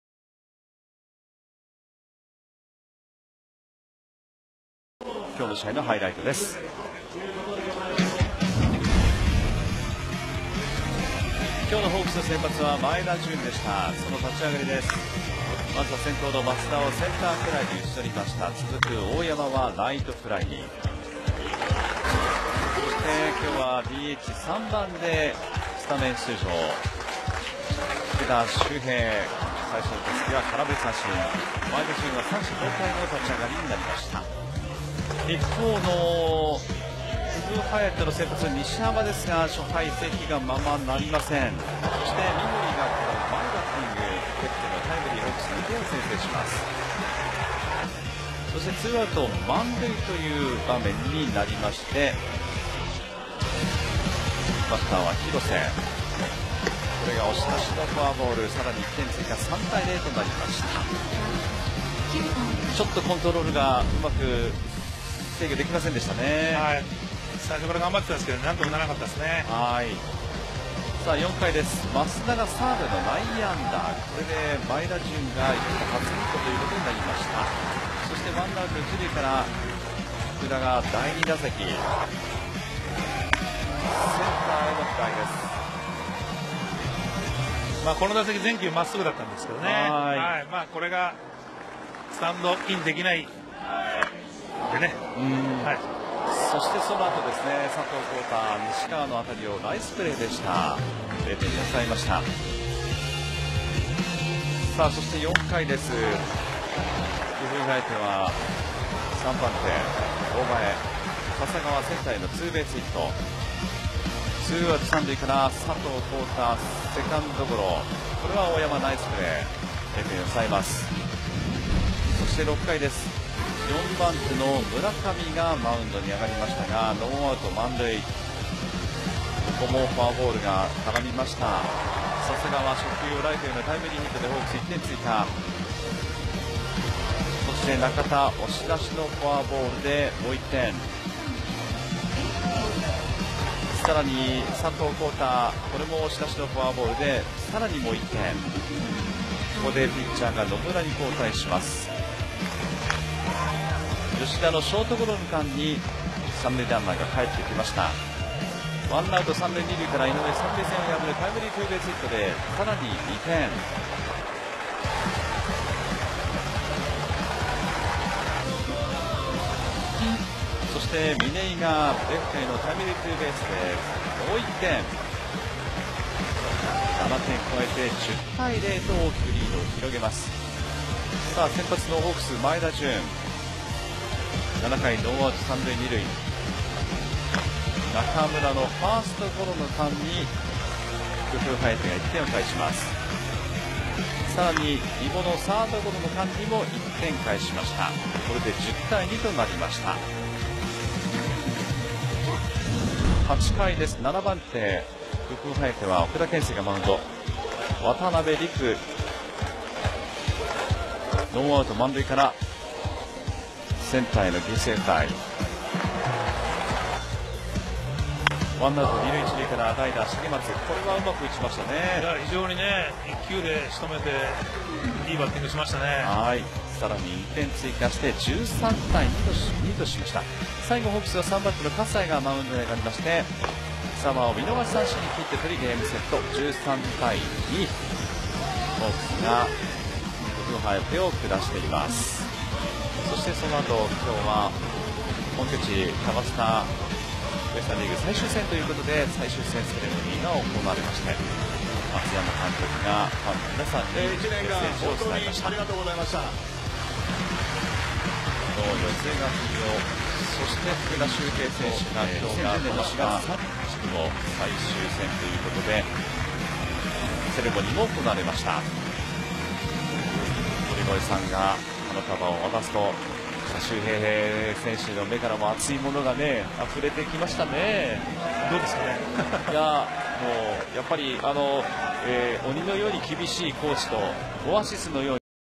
ファームハイライト】9月22 日 vsくふうハヤテ
H13-2V
ウエスタン・リーグ公式戦 9月22日 vsくふうハヤテ戦のハイライト動画です。